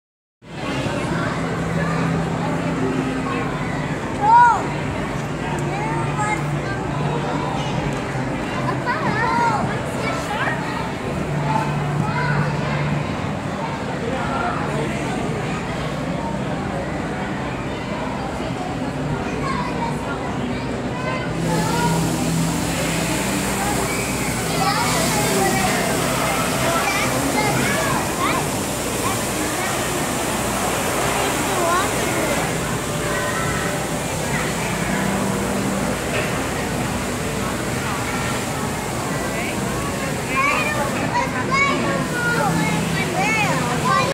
Ripley's Aquarium Toronto
The Cichlid tank at Ripley's. Nguyen family vacation.